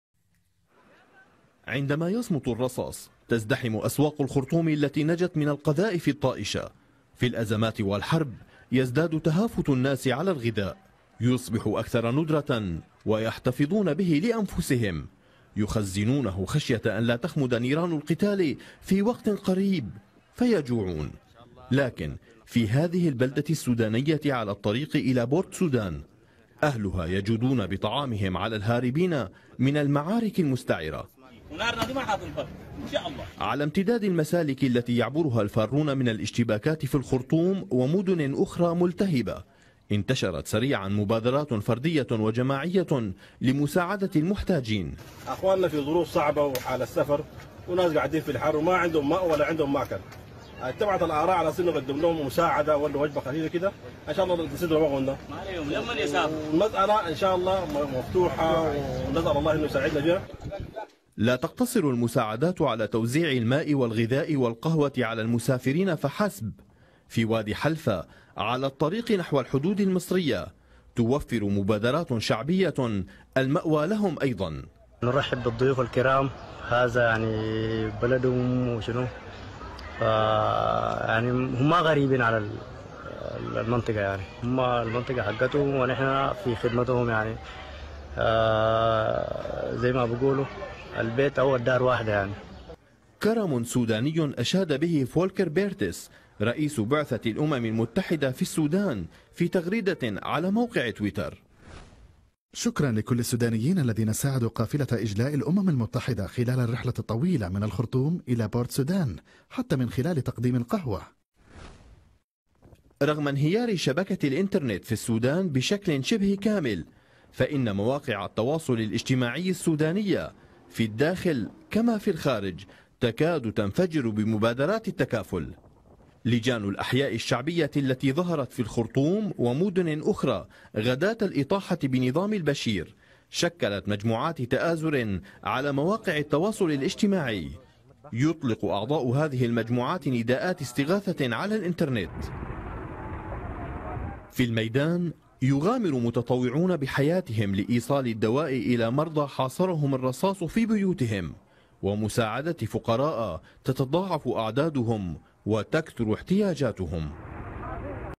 تآزر بين السودانيين ومبادرات لمساعدة المتضررين من الاشتباكات بتقديم الماء والغذاء والدواء لهم
مبادرات سودانية وتكاتف لمساعدة الفارين من الاشتباكات بتقديم الماء والغذاء والدواء لهم
تقرير: صابر أيوب